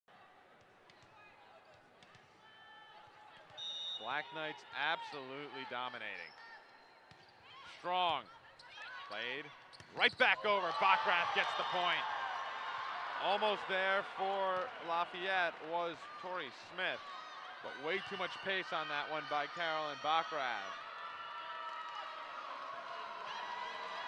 Army Volleyball: Carolyn Bockrath Kill vs. Lafayette 10-20-17
Carolyn Bockrath's relentless attack gives Lafayette no chance at a save as she sends a hard hit ball to the court for a Black Knights point.